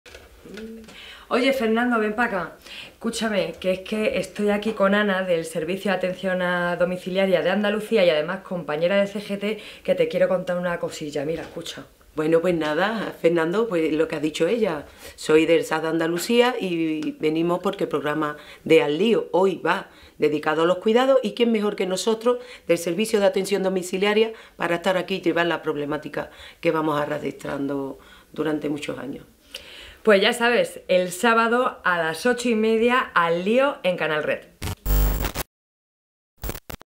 RNtv Promo 2 Al Lío
Próximamente segundo programa
Silvia Agüero Fernández, Ana Richarte, del Servicio de Atención Domiciliaria de CGT andalucia y ElEquipazo estarán el sábado 13 de mayo a las 20:30h en AL_LioTV para hablar de cuidados y de las que cuidan. En @canalredtv